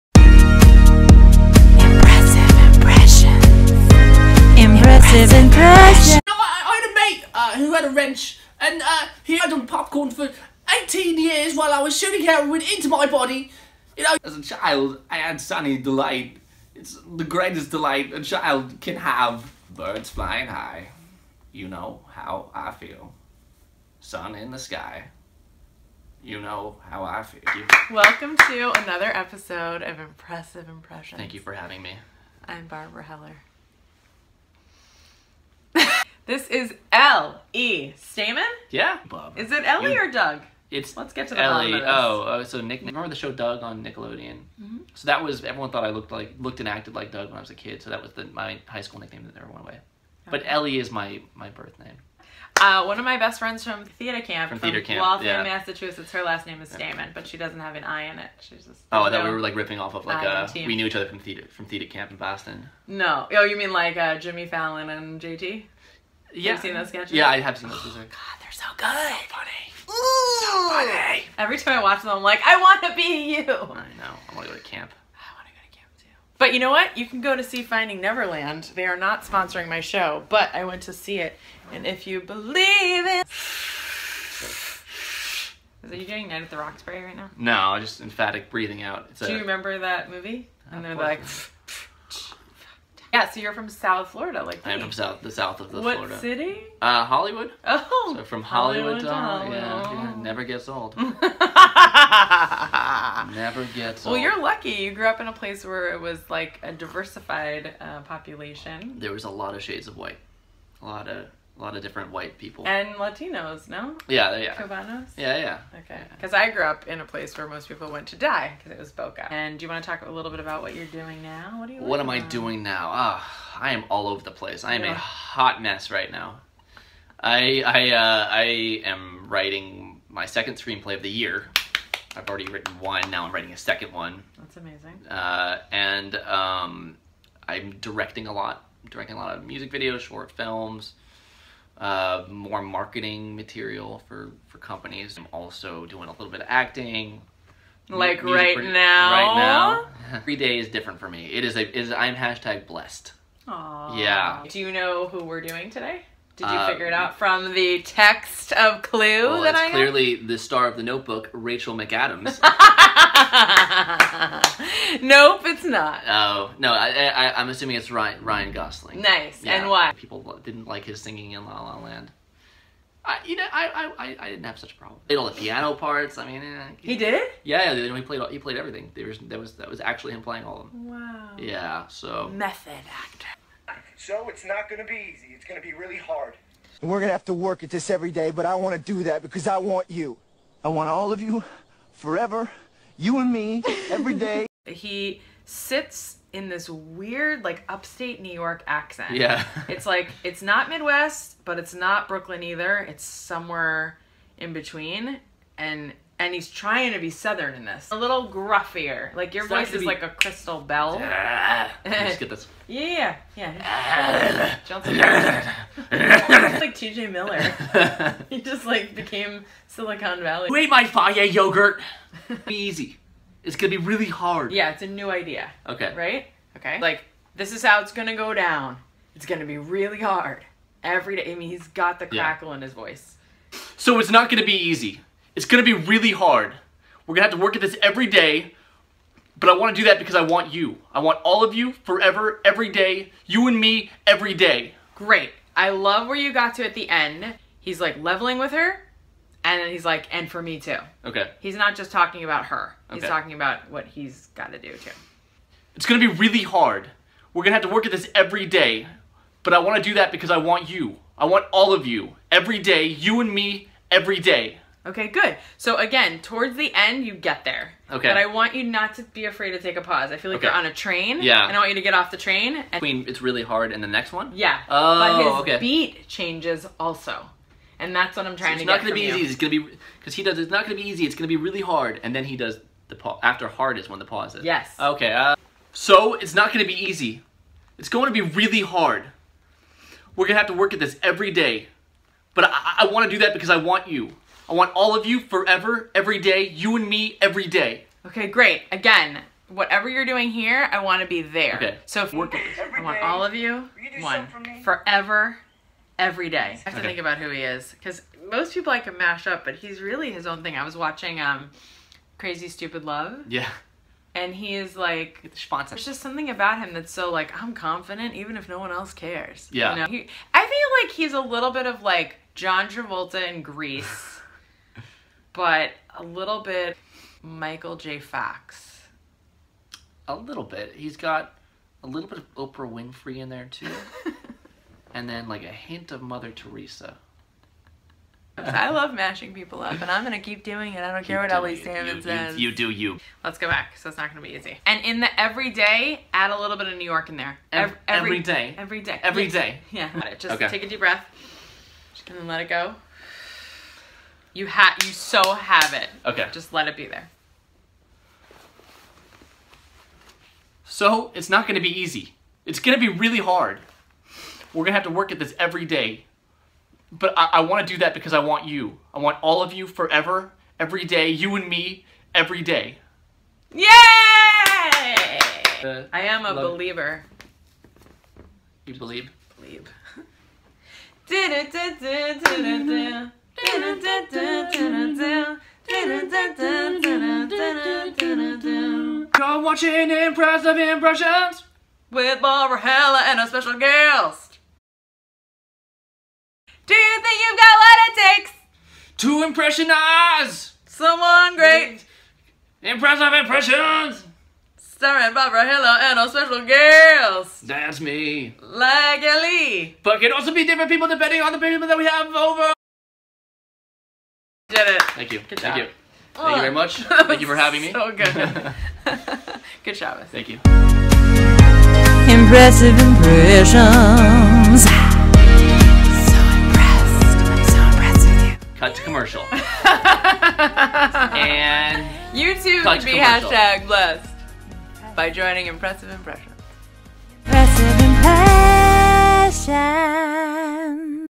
Impressive Impressions with Barbara Heller episode #8 starring L E Doug Staiman
Watch L. E. Doug Staiman master the impression of Ryan Gosling from The Notebook.